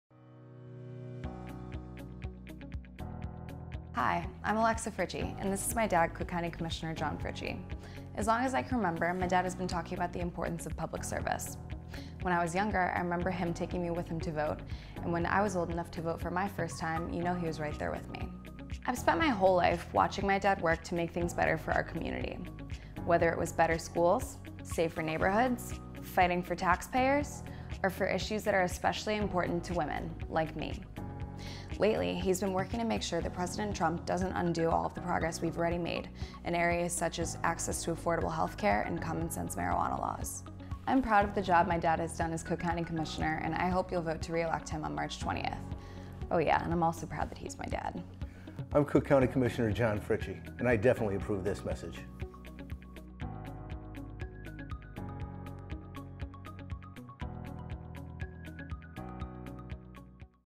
Let me tell you about my dad...
Over the next few weeks, I'll be announcing a number of endorsements for my re-election. And while it's an impressive list, this one means the most to me personally.

Please watch it and share it. Thanks.

- John